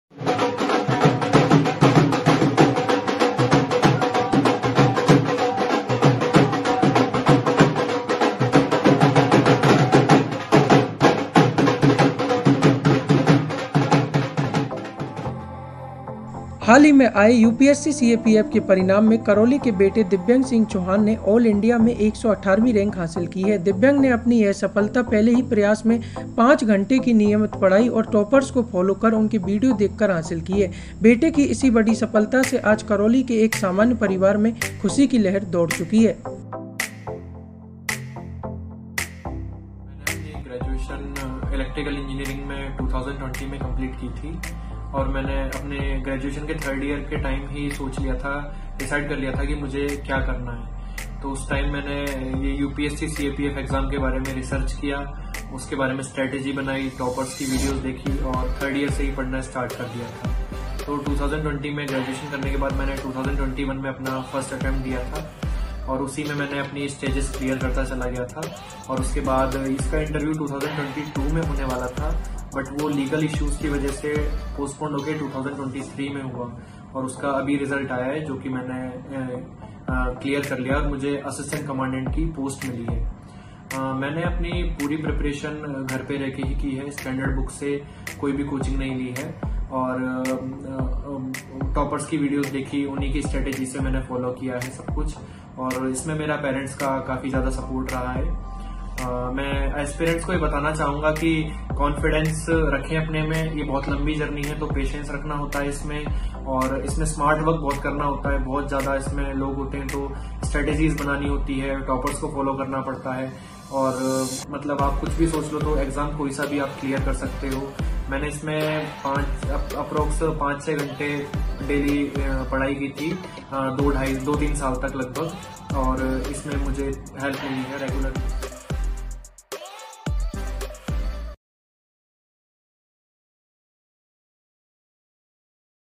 Success Story : दिव्यन ने CAPF में पहले प्रयास में पाई सफलता, 5 घंटे की सेल्फ स्टडी से मिला मुकाम
मोहित शर्मा/करौली. कहते हैं कि कुछ किए बिना जय जयकार नहीं होती, कोशिश करने वालों की कभी हार नहीं होती. इसी बेहतरीन कोशिश से जय जयकार हो रही है आज करौली के बेटे दिव्यन सिंह चौहान की. जिसने महज 24 वर्ष की उम्र में अपनी कड़ी मेहनत और दृढ़ संकल्प के चलते, हाल ही में आए यूपीएससी ( CAPF ) के परिणाम में ऑल इंडिया में 118 वीं रैंक हासिल की है. दिव्यन की सफलता की सबसे खास बात यह है कि उसने घर पर ही 5 घंटे की नियमित पढ़ाई से पहले प्रयास में ही कठिन और प्रतिस्पर्धी परीक्षा कही जाने वाली यूपीएससी ( CAPF ) में असिस्टेंट कमांडेंट के पद पर सफलता पाई है.

दिव्यन सिंह चौहान का परिचय
यूपीएससी ( CAPF ) 2021 के परिणाम में पहले प्रयास में 118 वीं रैंक हासिल करने वाले 24 वर्षीय दिव्यन सिंह चौहान करौली के तीन बड़ चौराहे की निवासी है. उनके पिता राजेंद्र सिंह चौहान शिक्षा विभाग में सहायक प्रशासनिक अधिकारी पद पर कार्यरत है. और उनकी मां ज्योति कंवर ग्रहणी हैं.

ग्रेजुएशन के लास्ट ईयर में सोचा था CAPF में जाने का सपना
दिव्यन का कहना है कि मैंने अपनी ग्रेजुएशन इलेक्ट्रॉनिक इंजीनियरिंग में 2020 में पूरी की थी. ग्रेजुएशन की थर्ड ईयर के टाइम में ही मैंने यूपीएससी ( CAPF ) में जाने का मूड बना लिया था. जिसके बाद मैंने इस एग्जाम के बारे में समझा और टॉपर्स की वीडियो देखने के बाद लास्ट ईयर में ही इसकी तैयारी शुरू कर दी थी. उनका कहना है कि 2020 में ग्रेजुएशन कंप्लीट करके 2021 में पहला एग्जाम दिया था.

एग्जाम क्लियर होने के बाद इसका इंटरव्यू 2022 में होने वाला था. लेकिन वह इंटरव्यू कानूनी विवाद के चलते आगे बढ़कर 2023 में हुआ. जिसका अब रिजल्ट आने के बाद मुझे असिस्टेंट कमांडेंट की पोस्ट मिली है.

घर पर रहकर ही की थी तैयारी
दिव्यन के मुताबिक उन्होंने अपनी इस एग्जाम की तैयारी घर पर रहकर बिना किसी कोचिंग के स्टैंडर्ड बुक के माध्यम से की थी. वह बताते हैं कि एग्जाम की तैयारी के वक्त में नियमित 5-6 घंटे की पढ़ाई में टॉपर्स की वीडियो भी देखता था. टॉपर्स की वीडियो देखने से भी मुझे बहुत मदद मिली. तैयारी के इस सफर में मेरे माता-पिता का भी मुझे काफी सपोर्ट रहा है.

यूपीएससी ( CAPF ) एग्जाम की तैयारी कर रहें युवाओं के लिए दिव्यन का कहना है कि अपना एक दृढ़ विश्वास रखें. लंबा सफर होने के कारण इस एग्जाम के लिए धैर्य रखना भी जरूरी है. मेरा तो सभी से यही कहना है कि CAPF एग्जाम में पोस्ट कम लेकिन कंपटीशन ज्यादा होने के कारण हर उम्मीदवार को स्मार्ट वर्क करना चाहिए वहीं टॉपर्स को भी फॉलो करके भी आप इस कठिन एग्जाम को अच्छे से समझ सकते हैं.